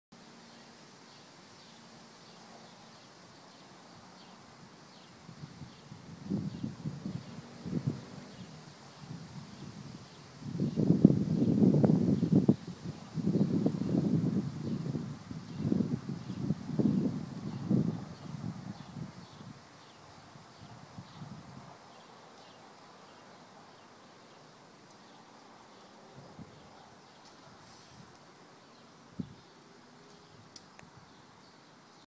Steak and Corn on the Cob Cooking on Grill
Steak and Corn on the Cob Cooking on the Grill
Made to just post to Facebook
I may or may not have been drinking Guinness at the time :)